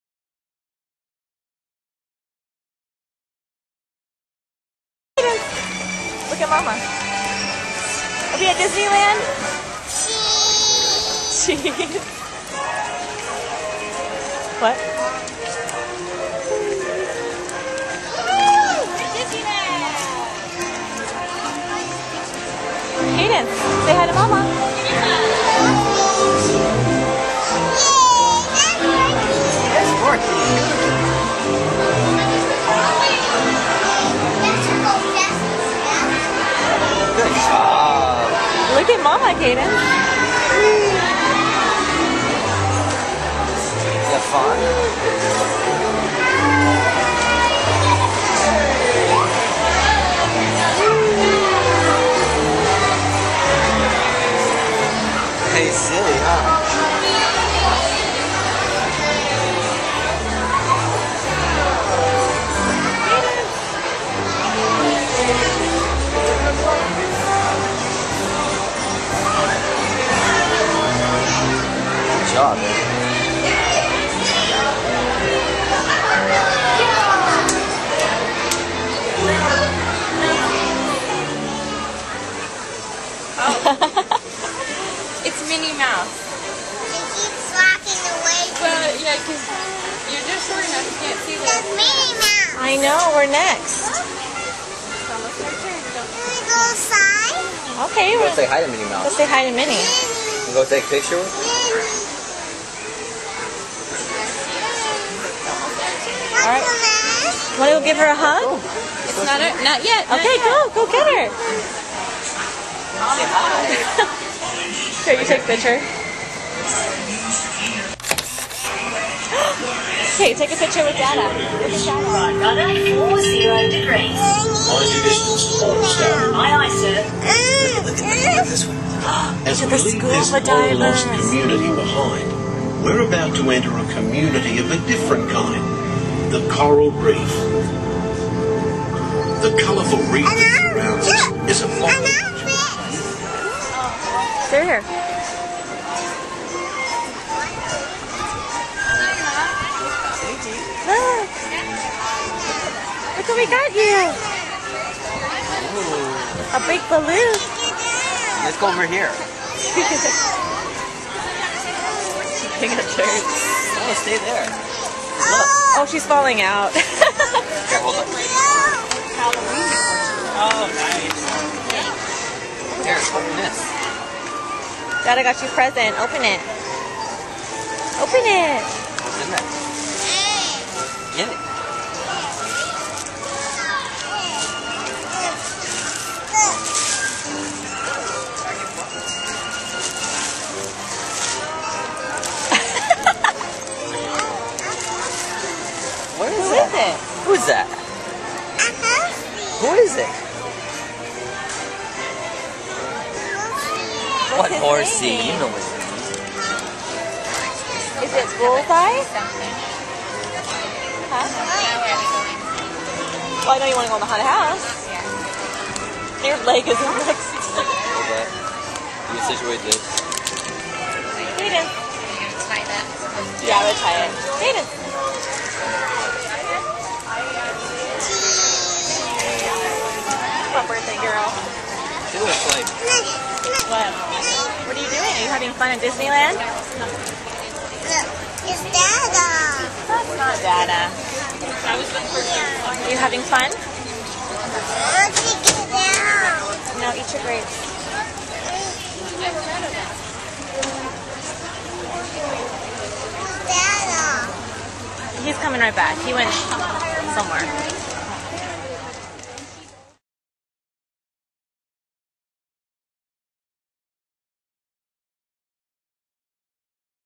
kaedynce movie.wmv
Kaedynce 3rd birthday at Disneyland